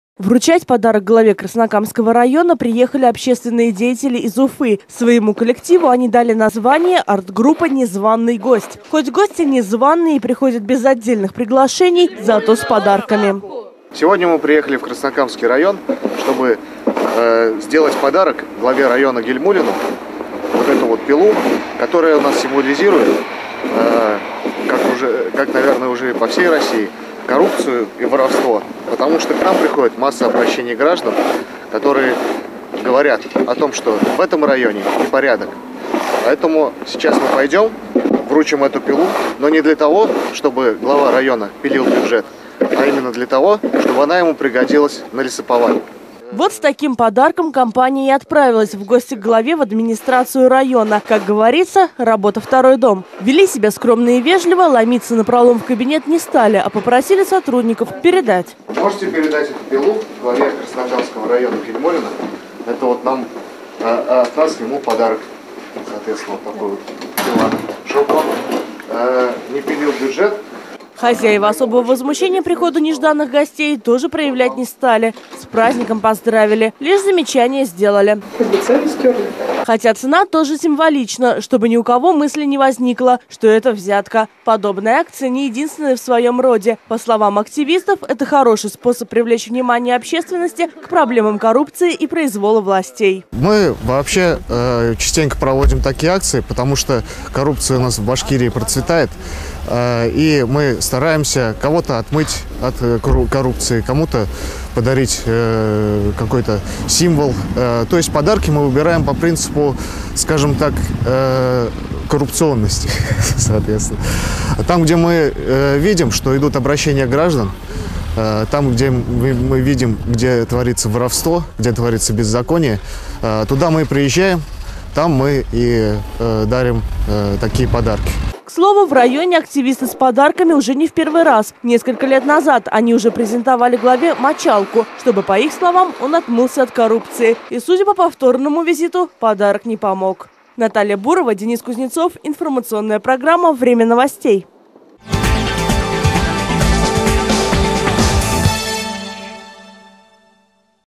«Тупая пила» как награда
Республиканские гражданские активисты нашли новый способ обратить внимание на проблемы. Если раньше в ход шли запросы и заявления в различные инстанции и правоохранительные органы, то теперь они начали дарить подарки. Один из символичных презентов достался и главе Краснокамского района Разифу Гильмуллину.